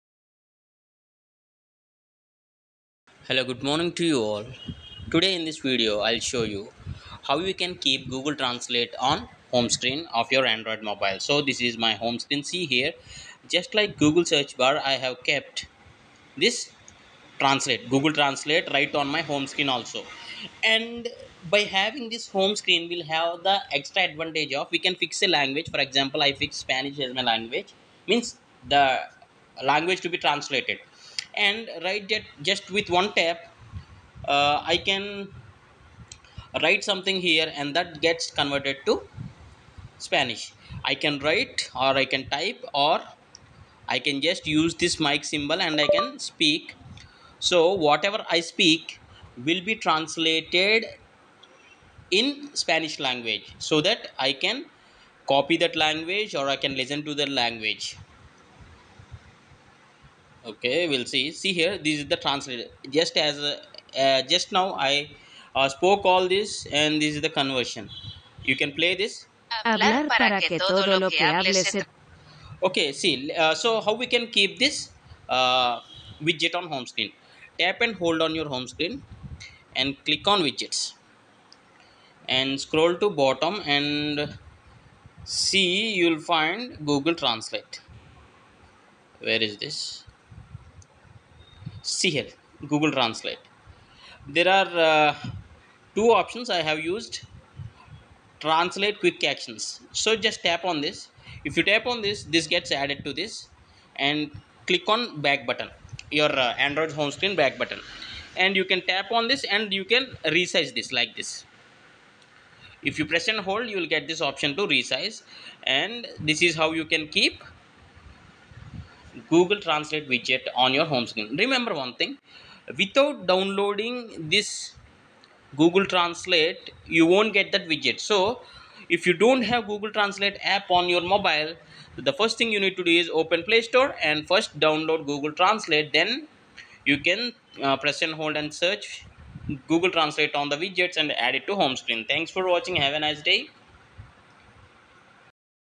How to Put Google Translate on HomeScreen
This is how you can easily put Google translate on home screen. Just like Google homework we can keep Google translate also on home screen of your mobile.

To do this initially we need to download Google translate app on our mobile then search for the Google translate widget. Dragon drop on the home screen to add to home screen.